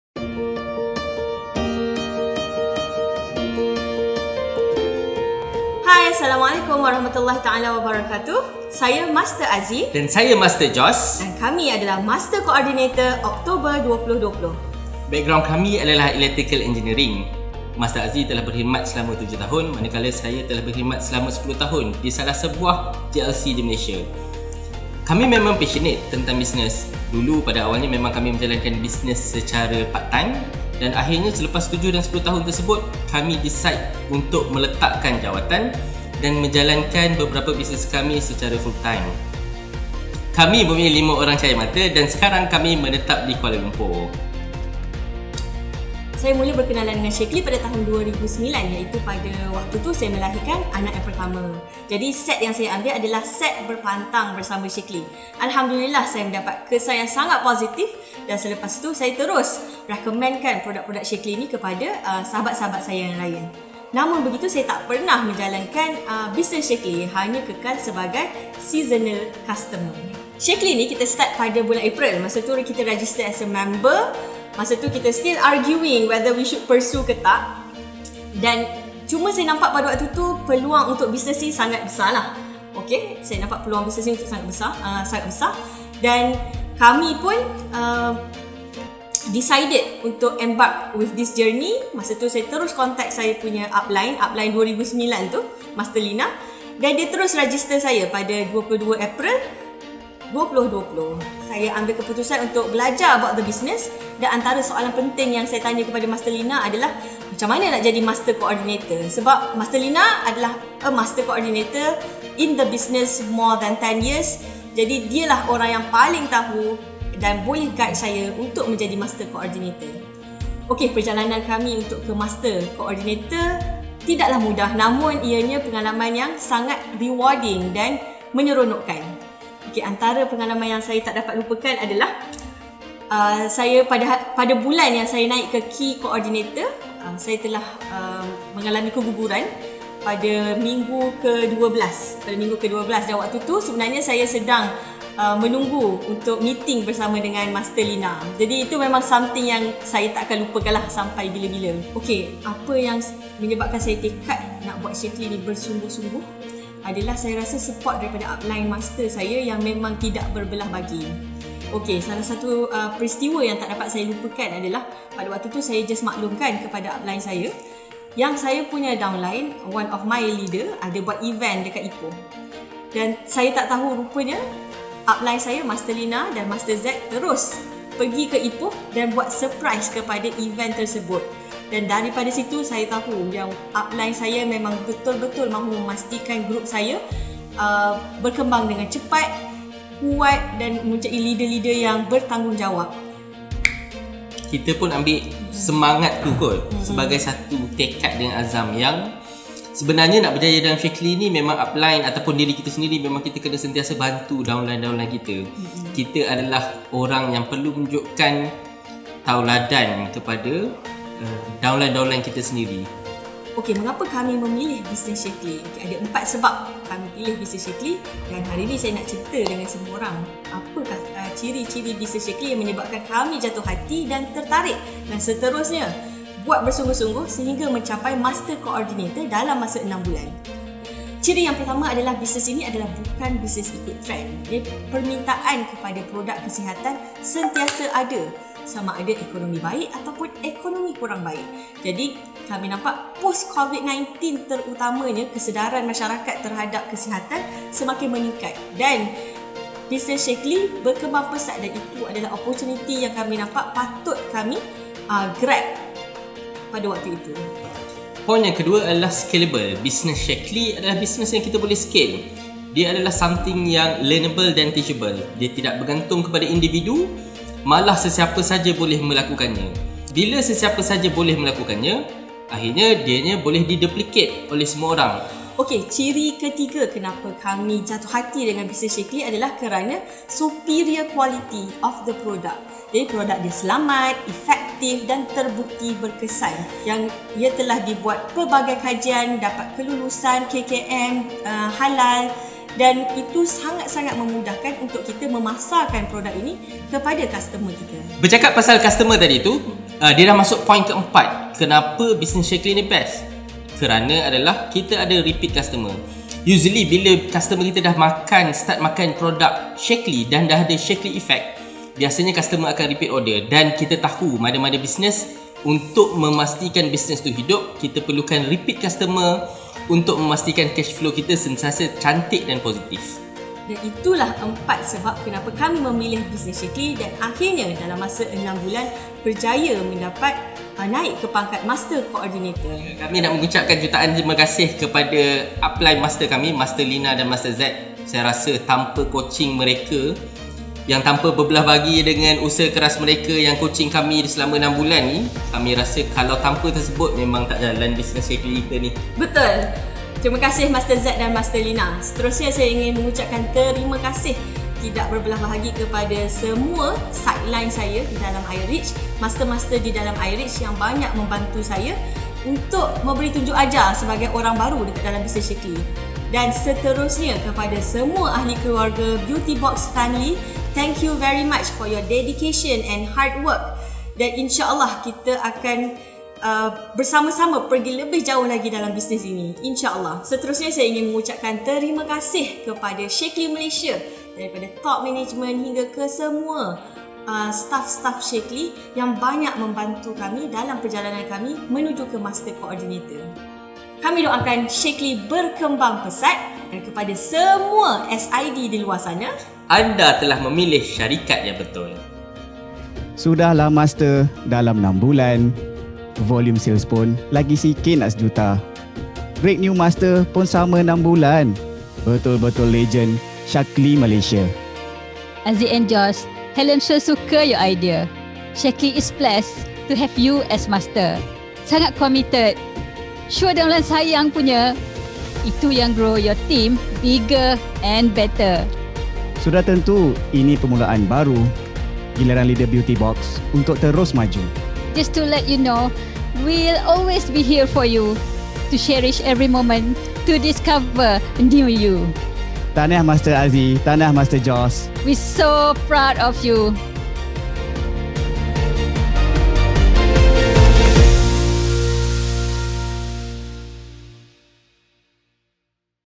Master Coordinators Nur Hafizah Shaarani & Muhammad Nizar Jamaludin
It took them much time and multiple businesses to know which is worthwhile. Once they embarked on the Shaklee journey, they never look back as it gave them the best in everything – financial freedom, flexibility and security that they never achieved with prior investments and businesses. Tune in to what makes Shaklee their lifelong career and how you can achieve the success that they do.